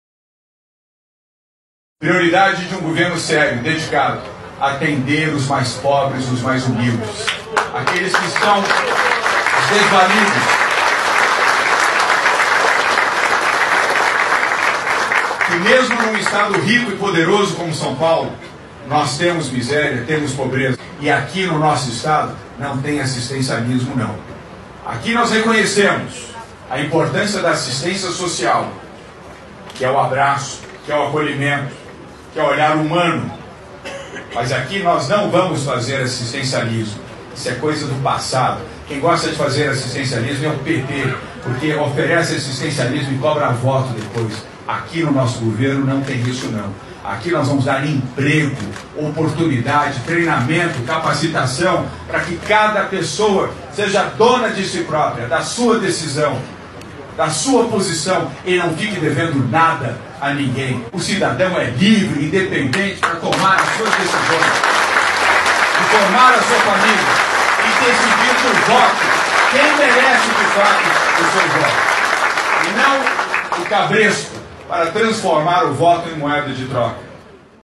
"QUEM GOSTA DE FAZER ASSISTENCIALISMO É O PT E COBRA VOTO DEPOIS", diz governador João Doria
"Chega de assistencialismo! A população mais vulnerável precisa, sim, ser acolhida, mas é fundamental desenvolver políticas públicas que tenham uma porta de saída para a autonomia do cidadão. A verdadeira conquista da dignidade é por meio do trabalho", afirmou o governador João Doria (PSDB).